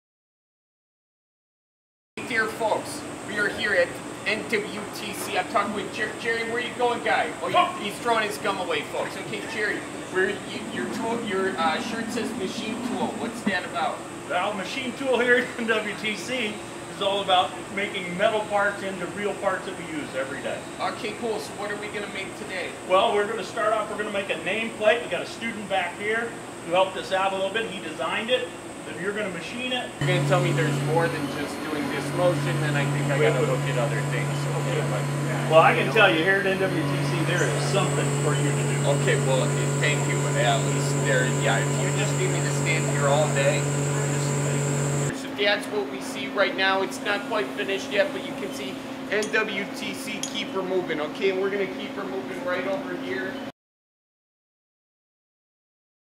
Charlie Behrens of the Manitowoc Minute visit NTWC Machine Tool
See what Charlie can do when he visit the NWTC Machine Tool program.